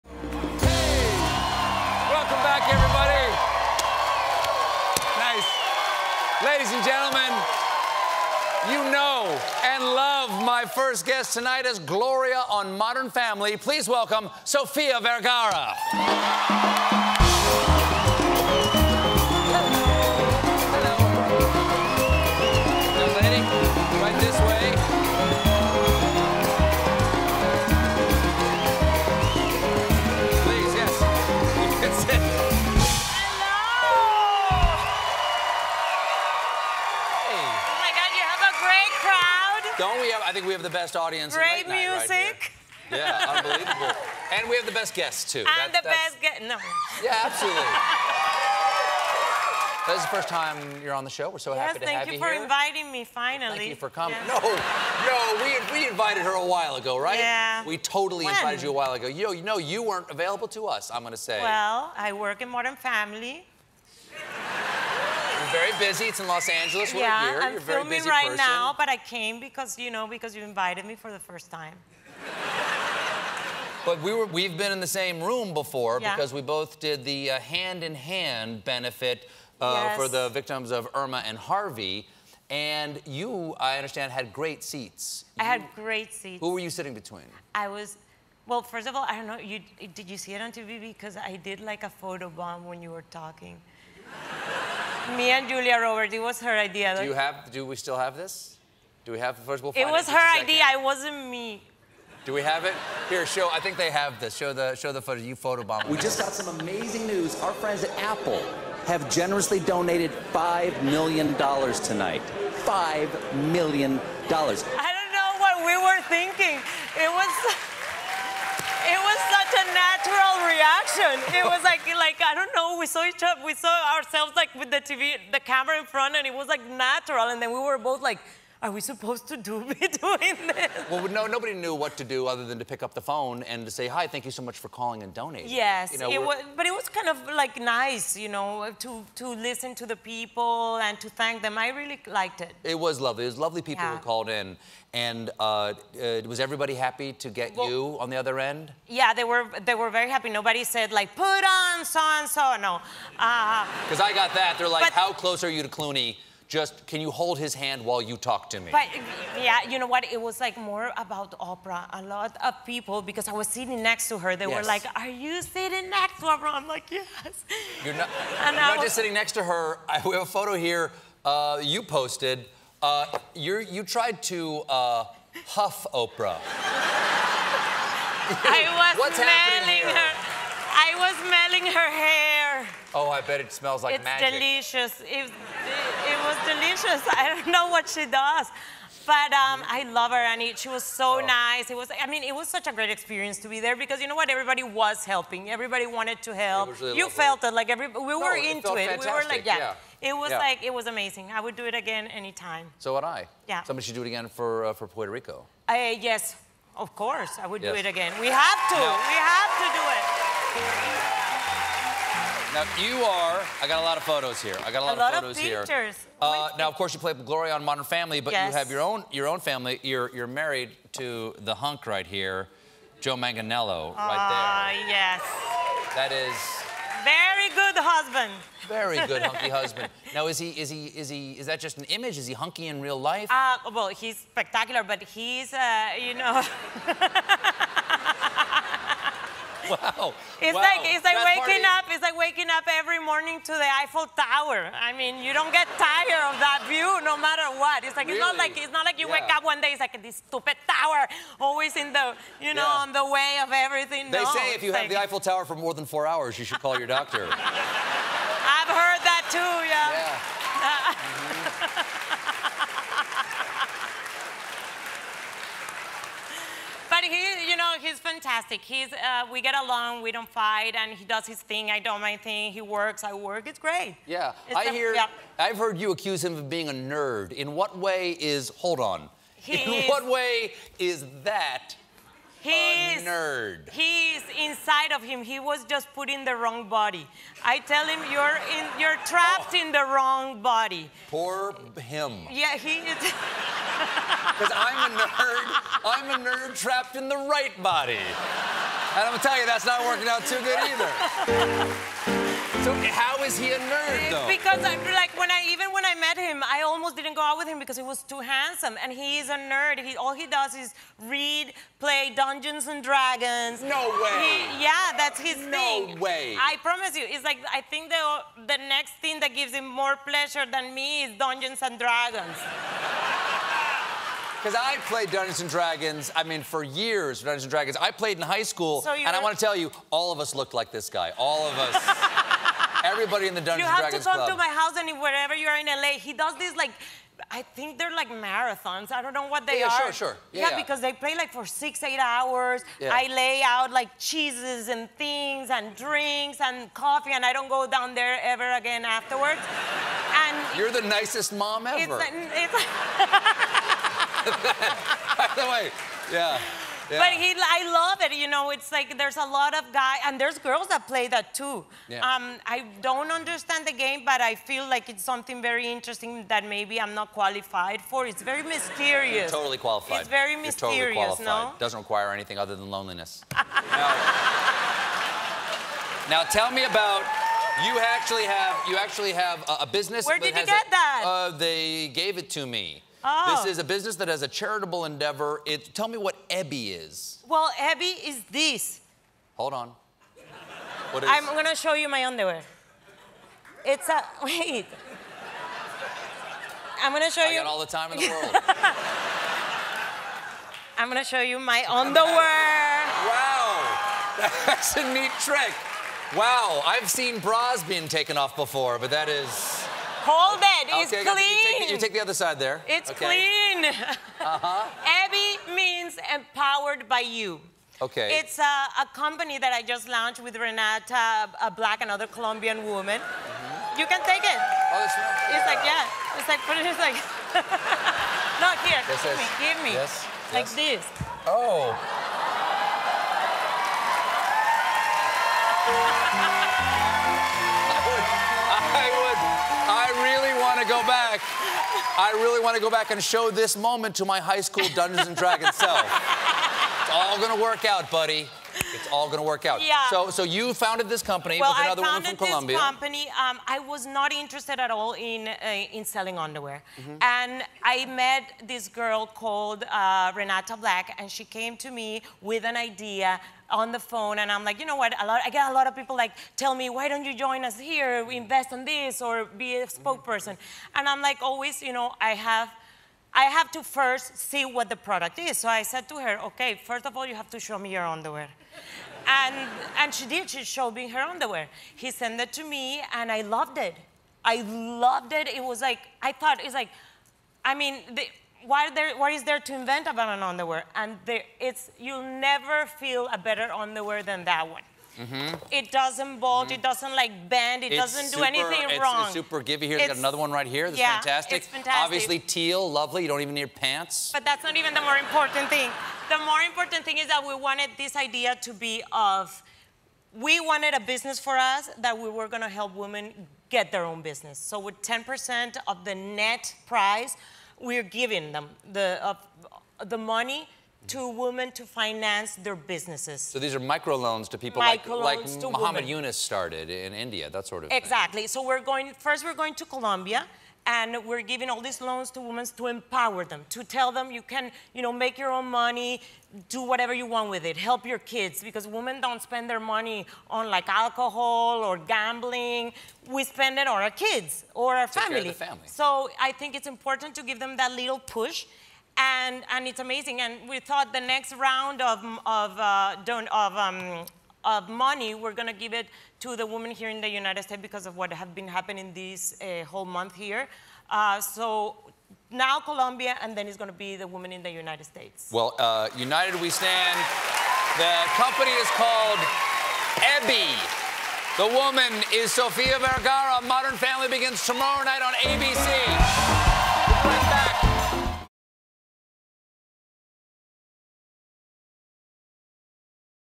Sofía Vergara Gives Stephen Her Underwear
'Modern Family' star Sofía Vergara has a present for Stephen. (Hint: she doesn't walk out with anything in her hands.)

Find out more about Sofia's mission to enable other women through business